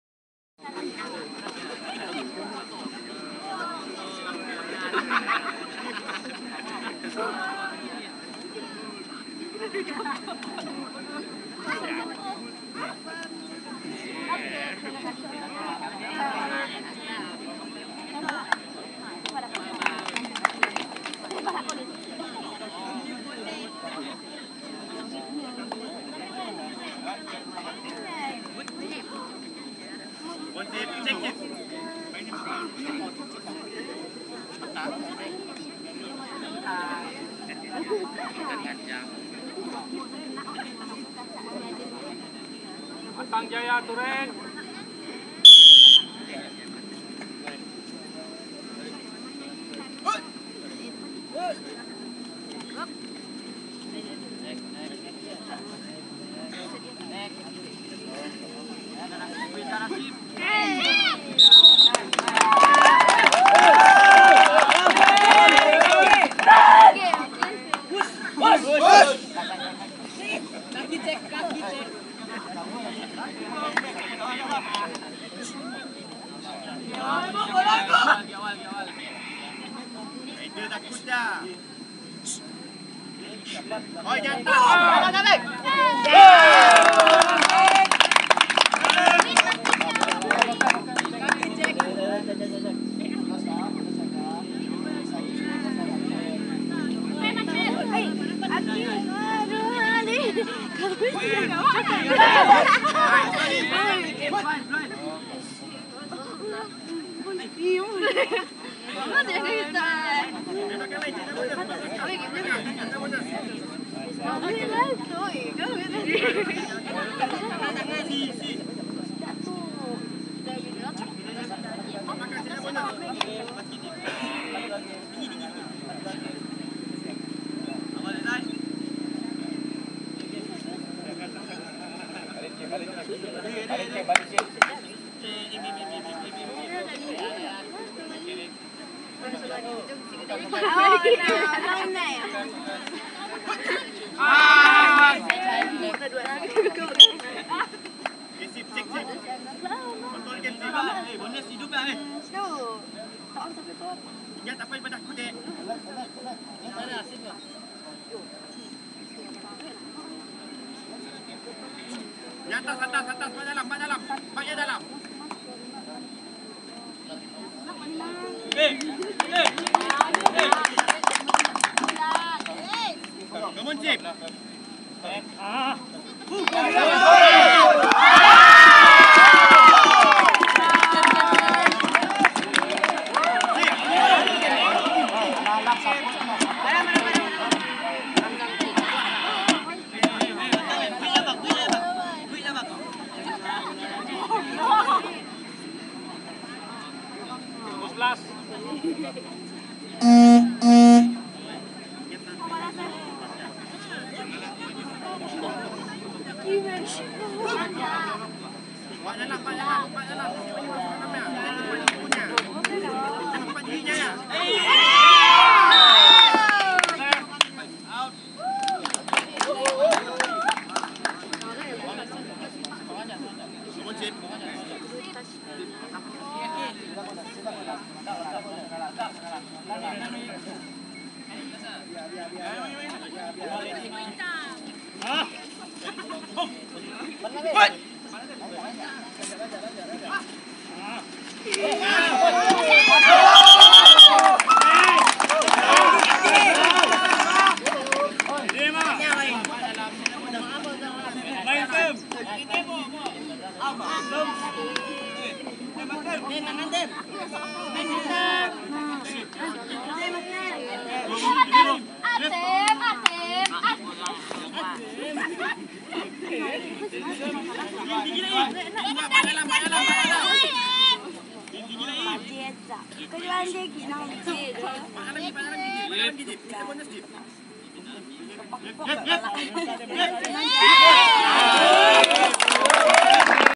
Perlawanan Separuh Akhir Kabaddi: SMK Matang Jaya vs SMK Tunaz
Perlawanan Kabaddi peringkat separuh akhir di antara SMK Matang Jaya dan SMK Tun Ahmad Zaidi (Tunaz).

MAKLUMAT VIDEO:
✪Lokasi video : Padang bola sepak SMK Matang Jaya
✪Peranti yang digunakan: Xiaomi Mi Note
✪Perisian editing: MS Movie Maker